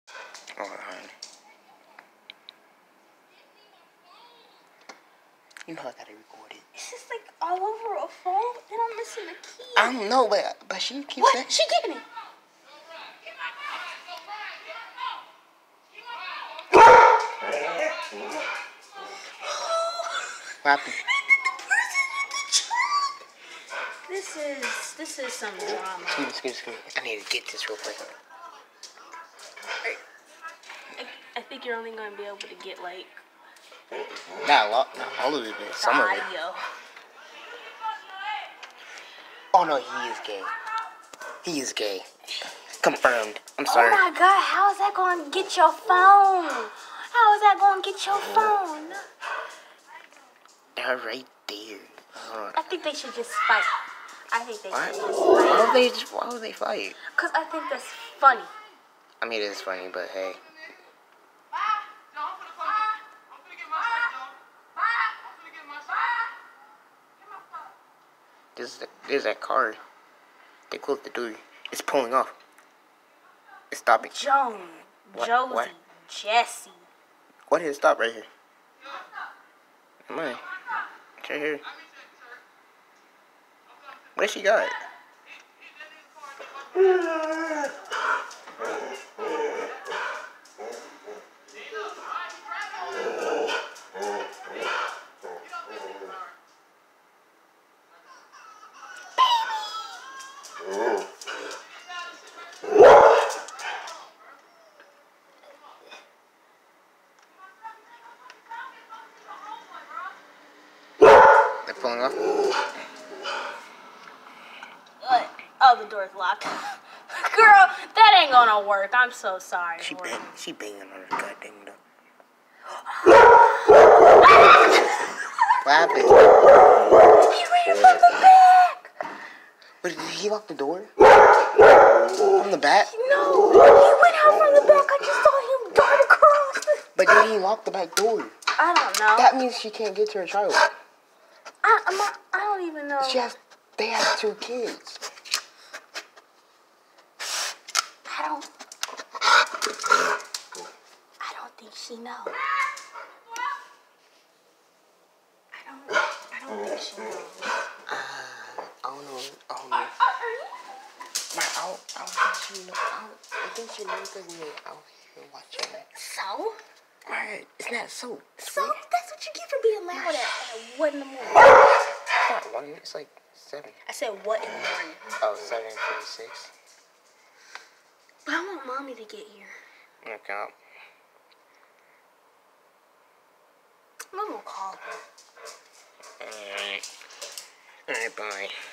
This couple was fighting outside and apparently he was secretly doing some sus stuff
guys don't cheat on yo partner